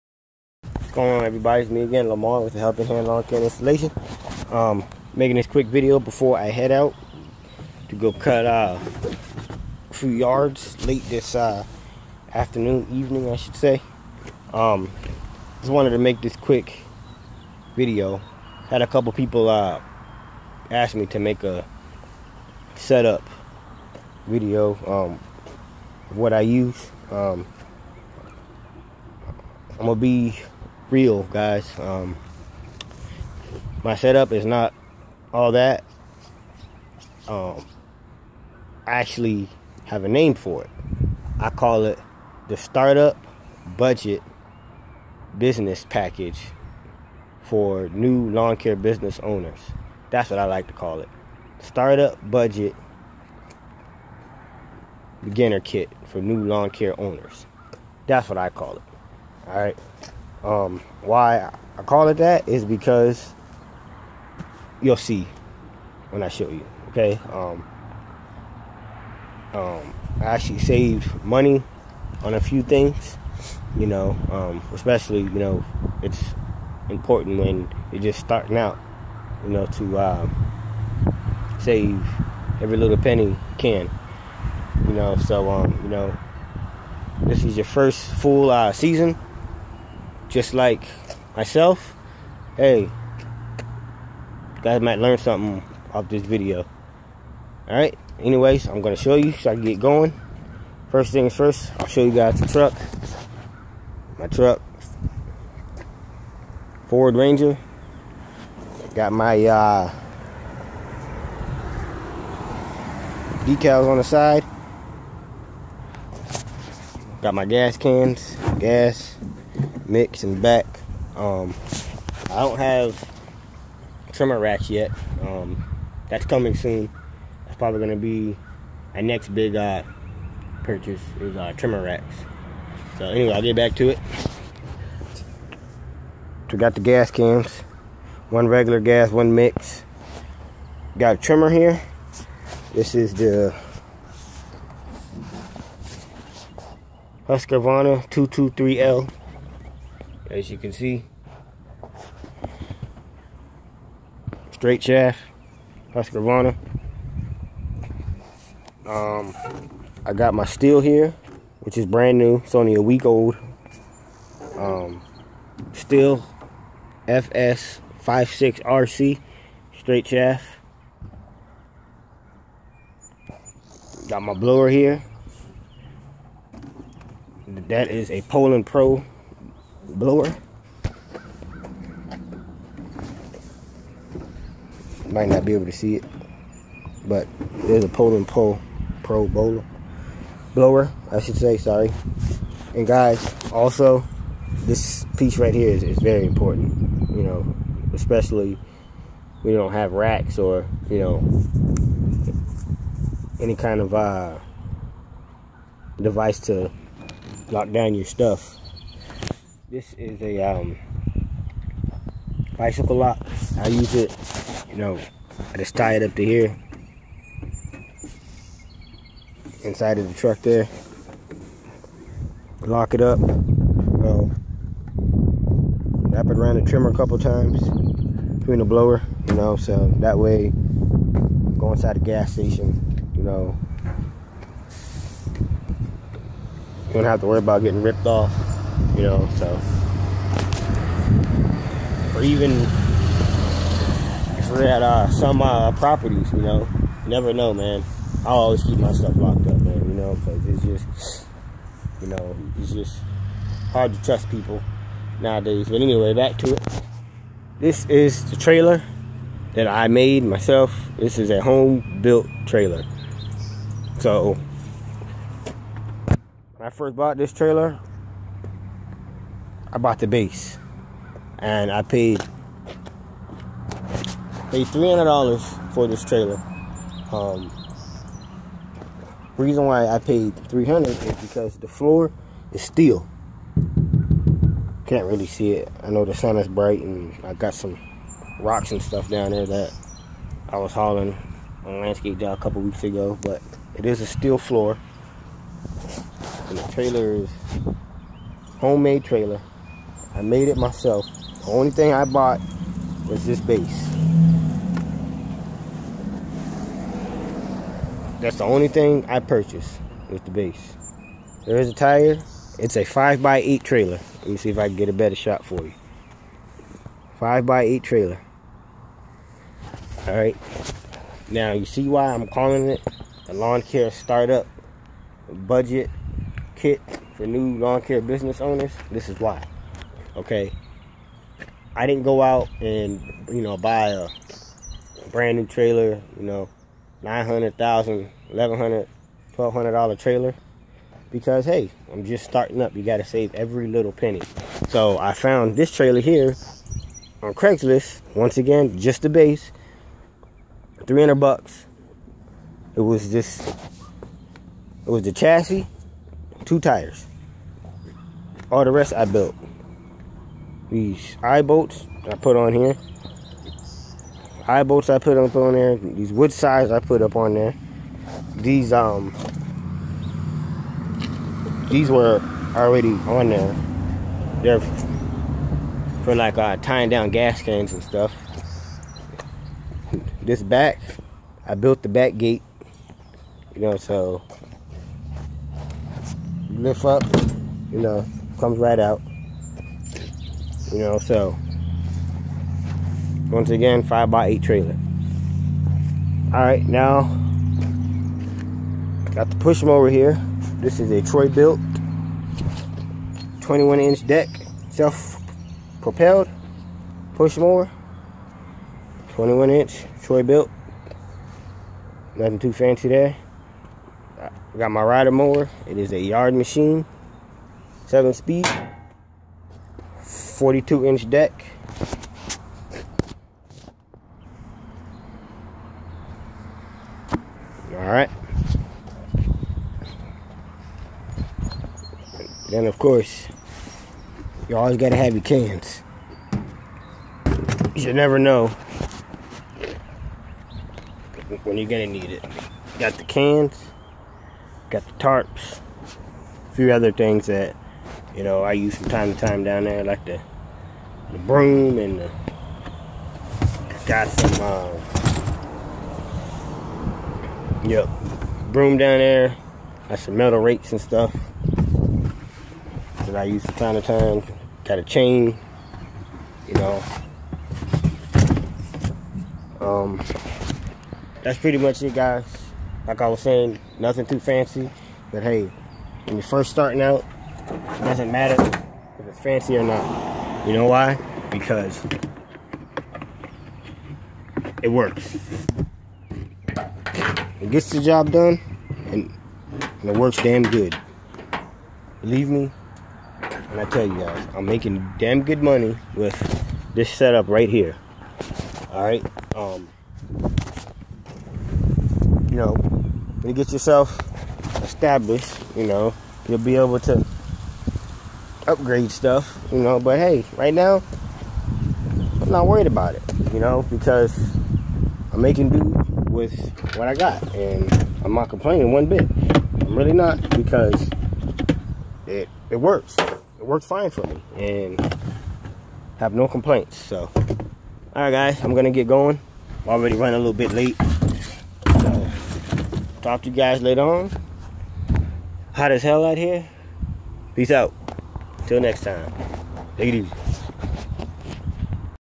My 2015 LawnCare budget package lawn care setup
Just a quick overview of my equipment for 2015 nothing special but i call it the budget package for new lawncare business owners. Facebook.com/ahelpinghandlawncare